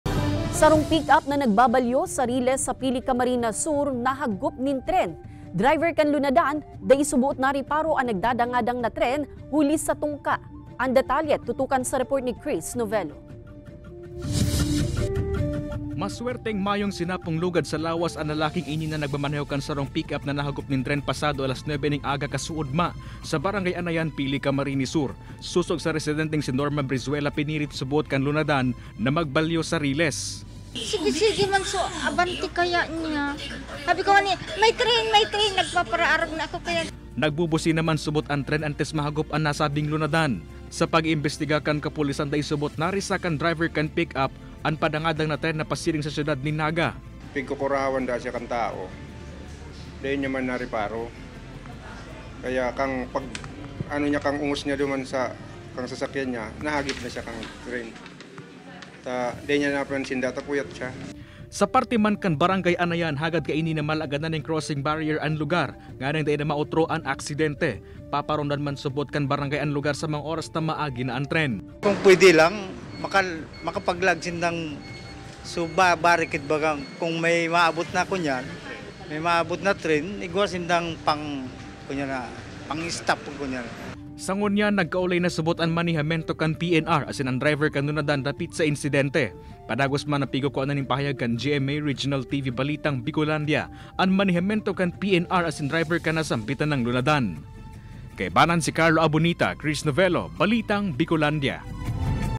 Balitang Bicolandia: Lunadan na nagbabalyo sa riles, nahagop nin tren
Lunadan na nagbabalyo sa riles, nahagop nin tren; Driver kan lunadan, dae suboot nariparo an papaabuton na tren huli sa tungka; Crossing barrier, hagad kan barangay sa PNR